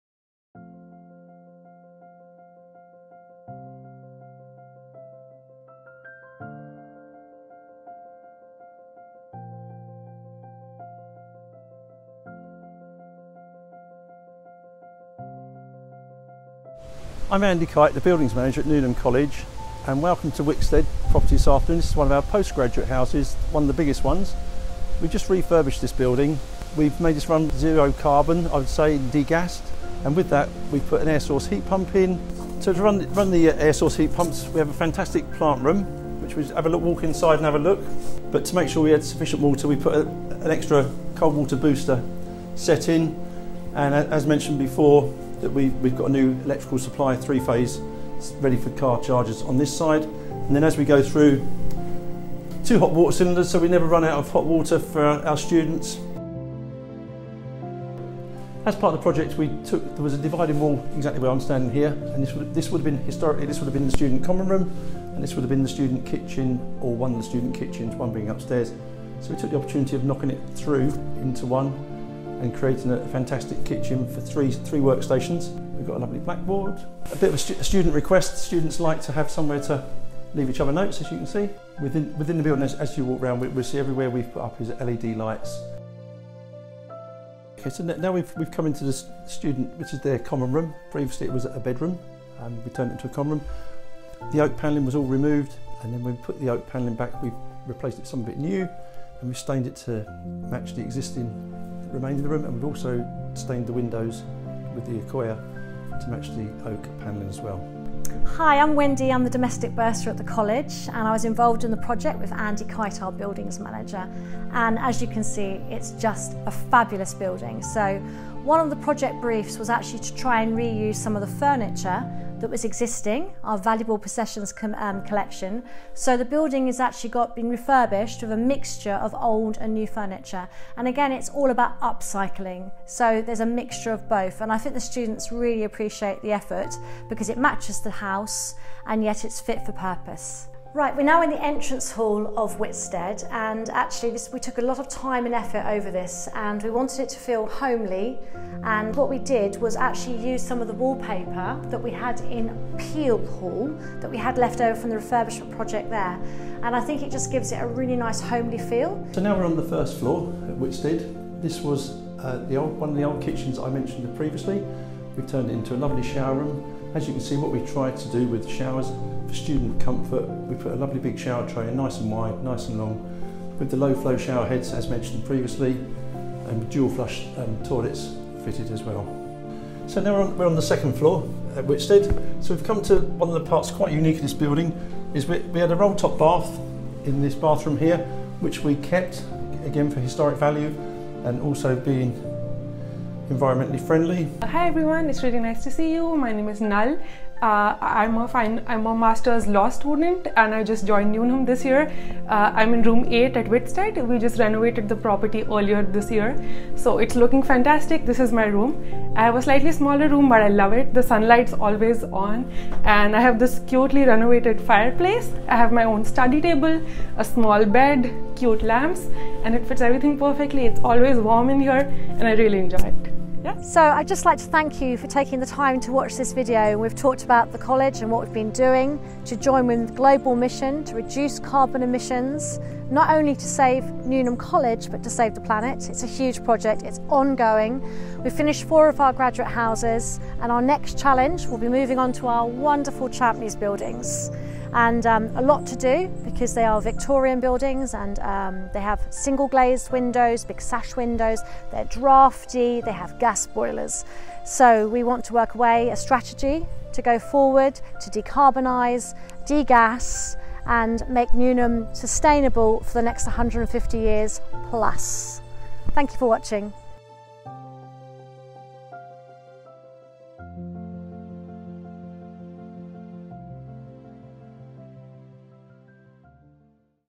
Shaping Newnham's Future - Greening Whitstead
We are building a £10 million fighting fund to green Newnham. Here's what we've achieved with our graduate house Whitstead.